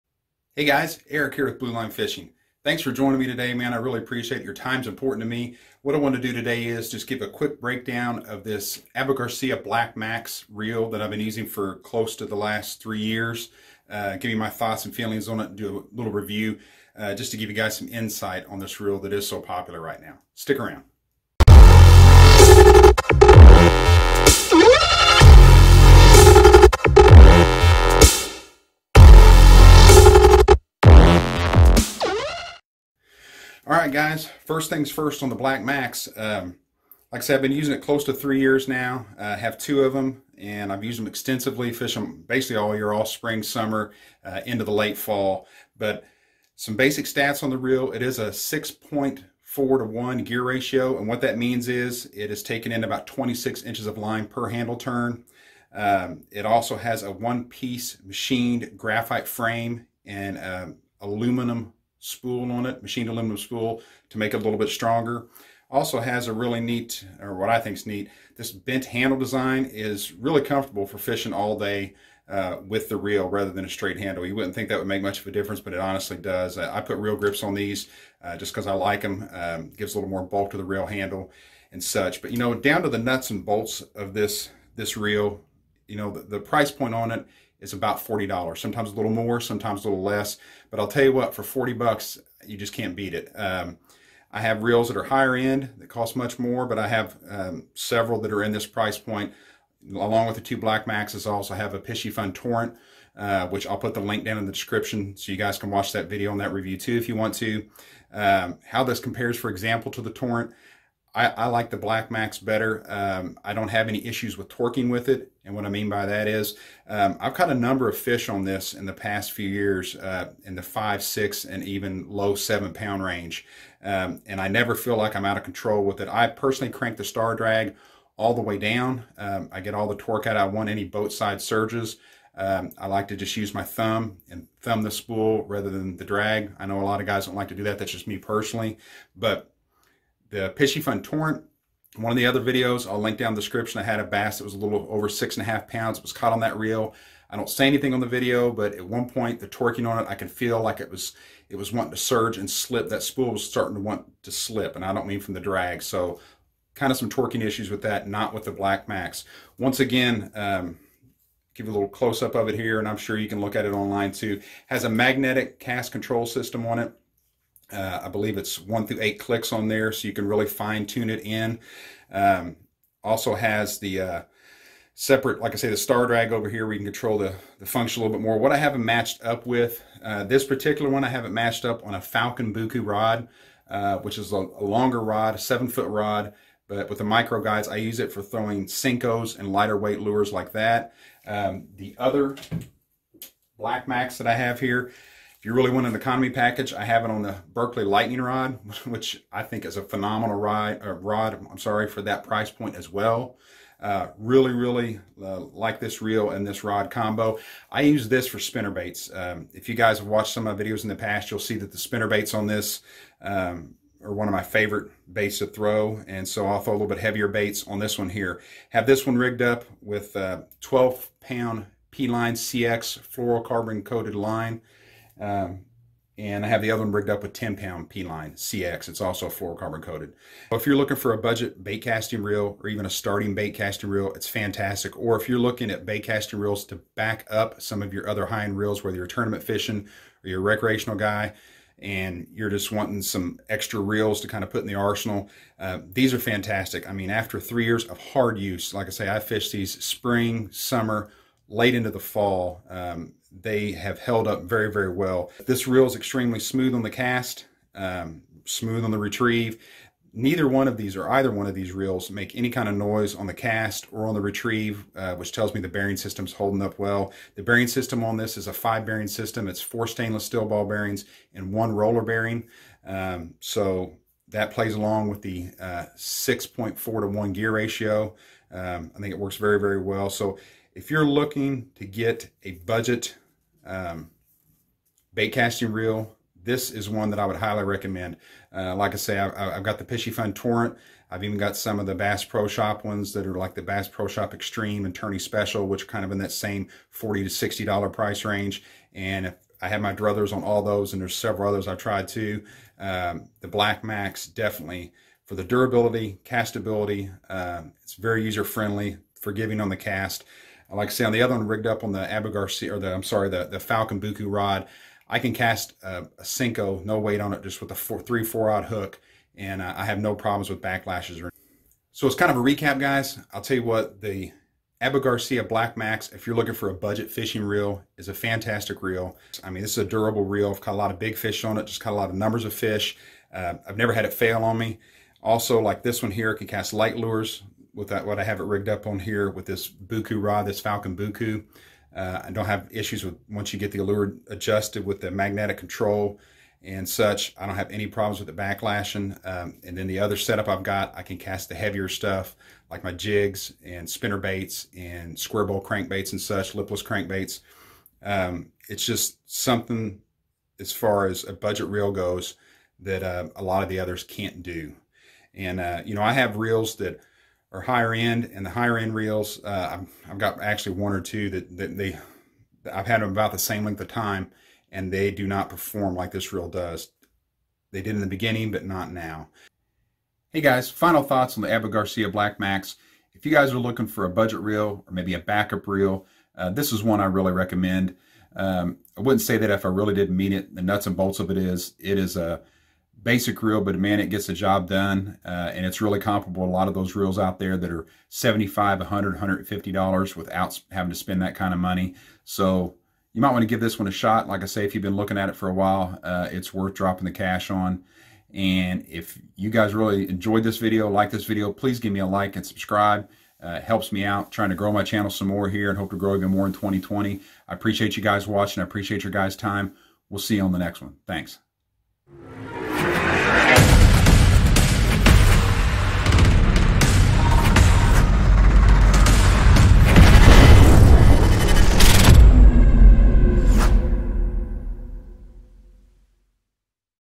Abu Garcia Black Max Review 3 Years Later/ Worth the Cash?
Abu Garcia Black Max Review After 3  Years Later. See how the popular Abu Garcia Black Max baitcasting reel held up after nearly 3 years of hard fishing use. Thinking of buy this fishing reel and been on the fence about it? Well check the video out before you do and see what the verdict is on this reel!
Blue Line Fishing 🎣 is on:
Abu Garcia black Max Reel Stats:
7.3 ounces
6.4-1 Gear Ratio (26 inches of line per handle turn)
4 Stainless Steel Ball Bearings + 1 Roller Bearing
Machined Aluminum Spool
Power Disk Drag System
Duragear Brass Gear
MagTrax Brake System (magnetic)
Compact Bent Handle Design
Recessed Reel Foot
One piece Graphite Frame
Graphite Sideplates
Abu Garica Black Max: